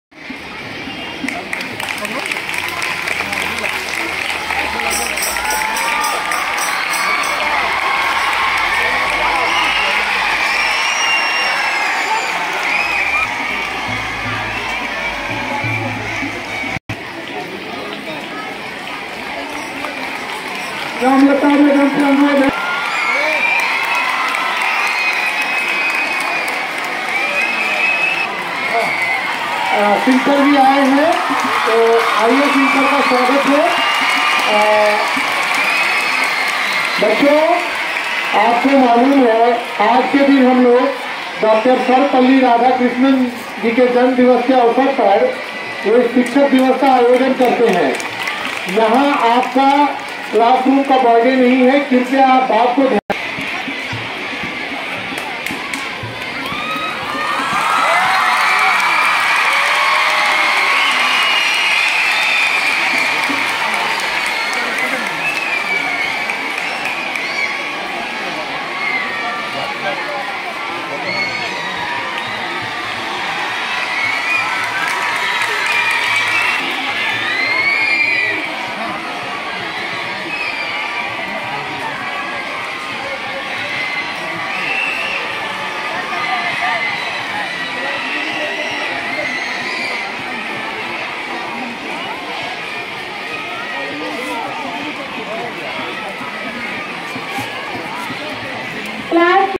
Teacher's Day Celebration on 05th September 2022
Deep Prajwalan Programme on Teacher's Day Celebration at KV Katihar.